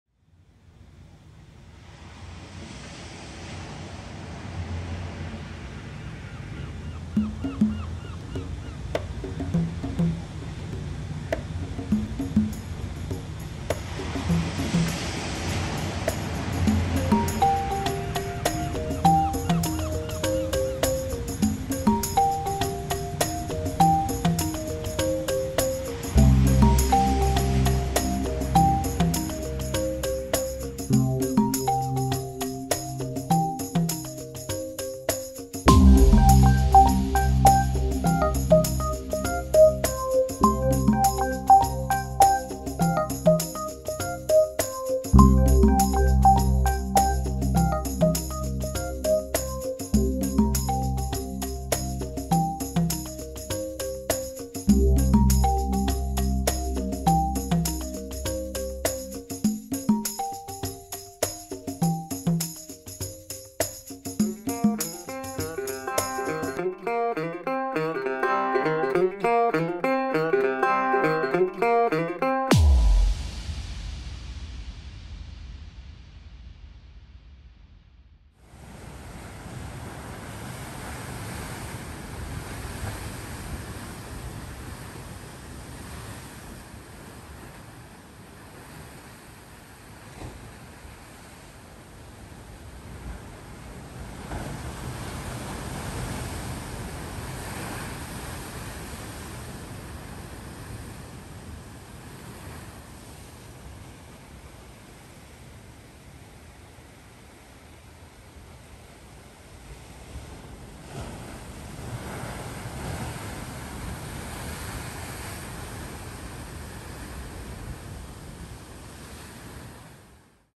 Our Hawaii Trip - 2022
Such a lovely place! A bit of the big island and a bit of Kauai... with original music from my little studio.